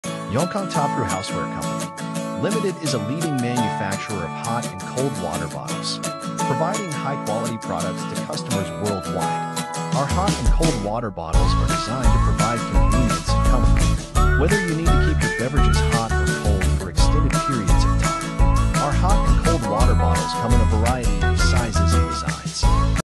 Hot And Cold Water Bottle Manufacturer
Yongkang Toptrue Houseware Co.. Ltd. is a leading manufacturer of hot and cold water bottles. providing high-quality products to customers worldwide. Our hot and cold water bottles are designed to provide convenience and comfort. whether you need to keep your beverages hot or cold for extended periods of time. Our hot and cold water bottles come in a variety of sizes and designs.